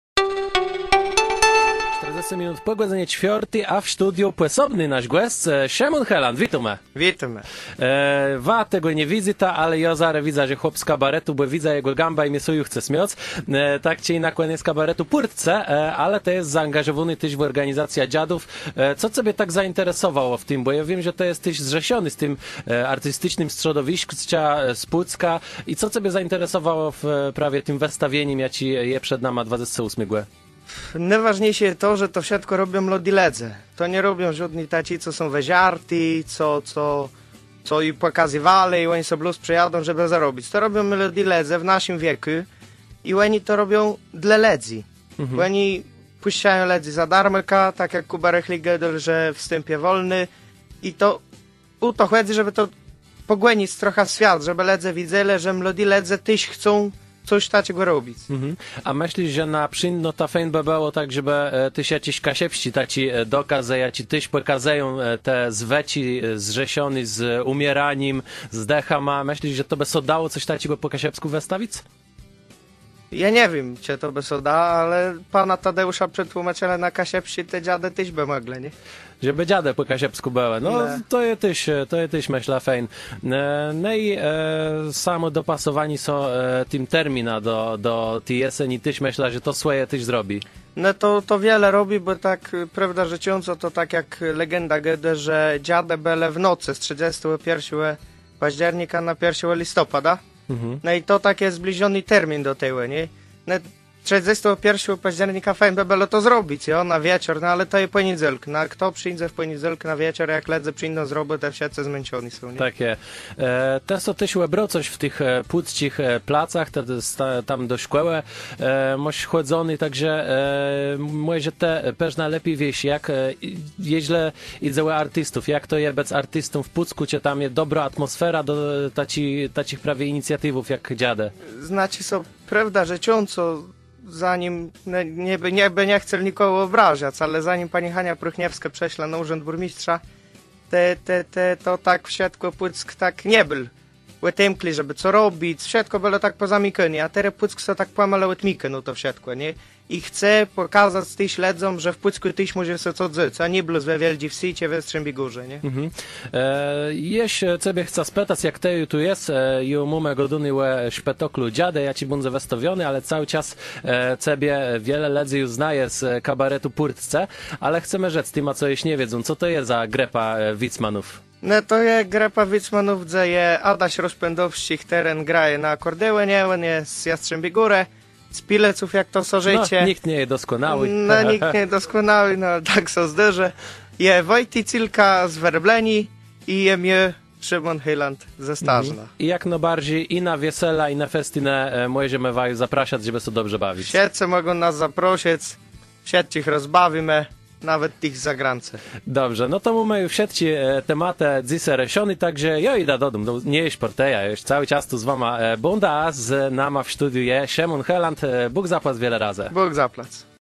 Szëmón Helãd – Dżadë Mickewicza
W Pùckù w Halë przë Skôrbòwim Ùrzãdze 28.10 aktorowie przedstawią Dżadë Mickewicza.
W wëdarzenié sã zaangażowôł téż znóny z kabaretu Pùrtcë Szëmón Helãd, jaczi je zadowòlniony z òdemkłoscë pùcczich wëszëznów dlô młodëch ùtwórców, a téż wërôżô nôdzejã, że Dżadë mdą pòsobnym pò Panu Tadeùszu Mickewiczowim dokazã na kaszëbsczi przełożonym